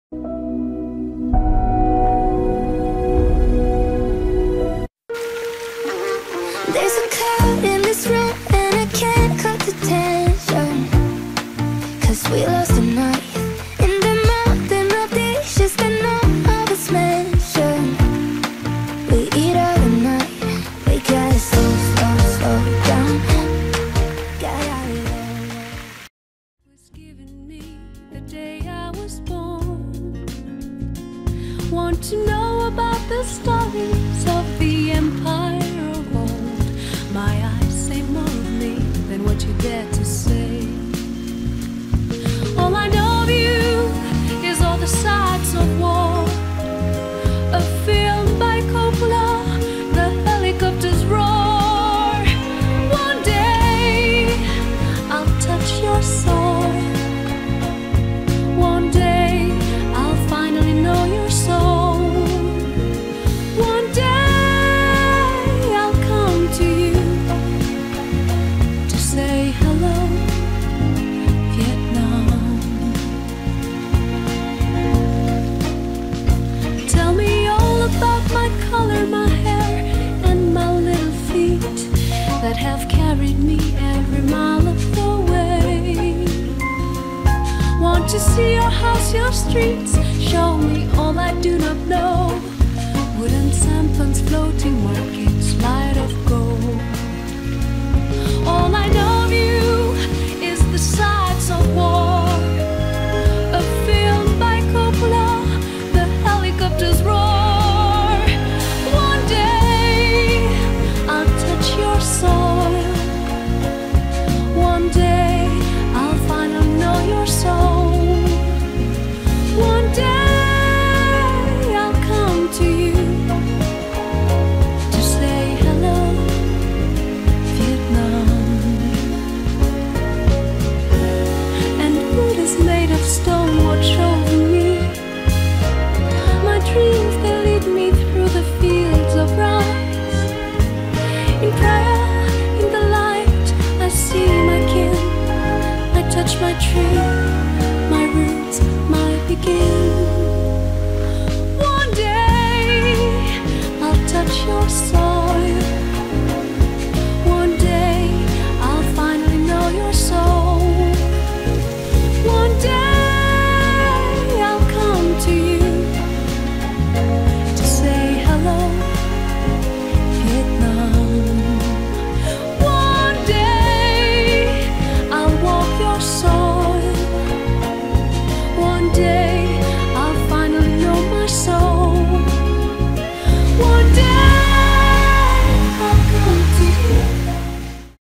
2D multidirectional nozzle - Musical fountain - Hello Vientnam. By iconvina.com
Underwater robot arm for musical fountain